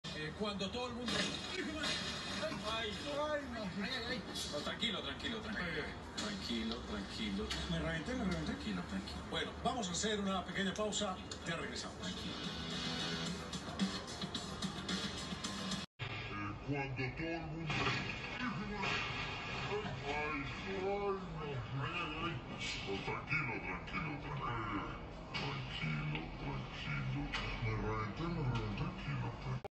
ESPN COLOMBIA ANCHOR GETS WRECKED!
Yikes. ESPN Colombia radio anchor gets absolutely F*CKED on LIVE TV by a falling piece of the set. Did they run out of screws or something? They use zip ties? Yikes.

Not sure if he's ok, but nonetheless, it didn't look very good.